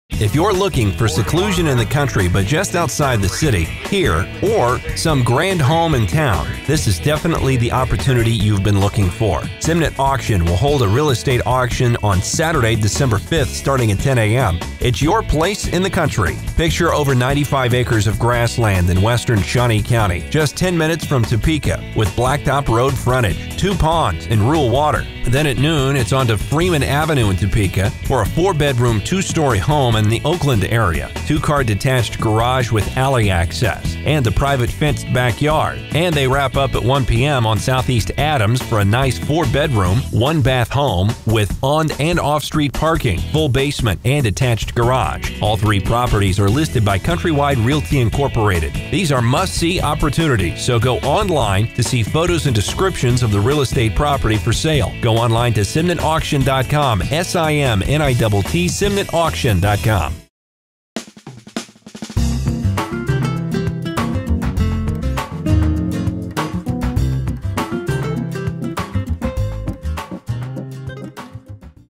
Simnitt Real Estate Auction December 5 2015
Three properties go to auction on Saturday, December 5, 2015 in and around Topeka Kansas.

Auctions by Simnitt Bros., Inc.